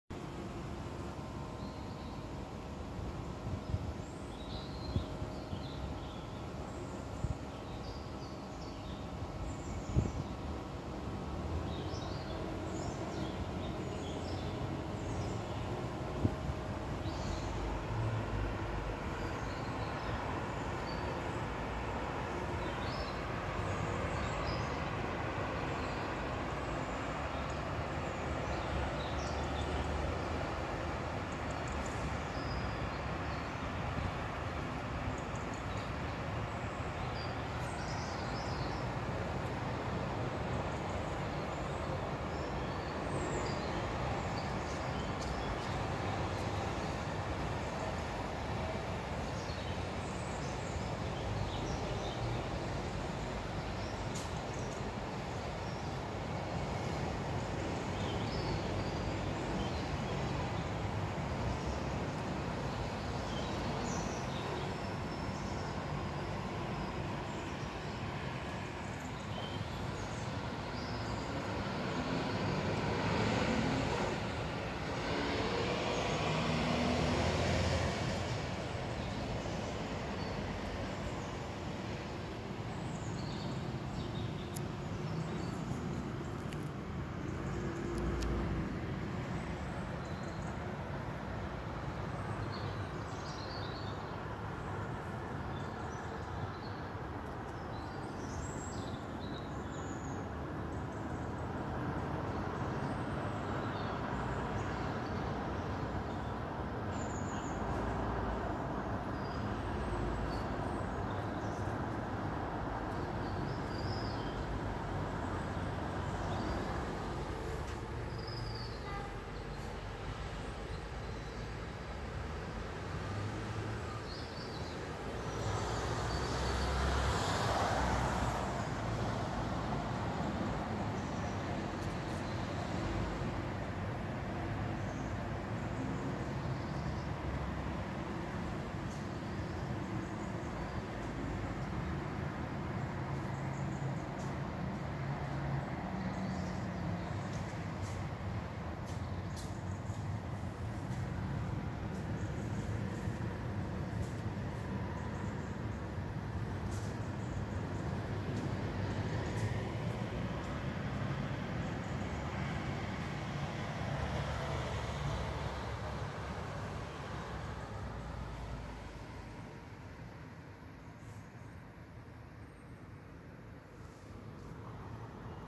Bee for Brackenbury 🐝FOR ARTIST AND SUFFRAGETTE GEORGINA BRACKENBURY 🐝
Filming videos for the first time from SuffragetteMouseCastleLondon the former home of artist GeorginaBrackenbury, today was about the bees on our ironingboardgarden and balconygarden in September. Just a bee, doing what bees do best.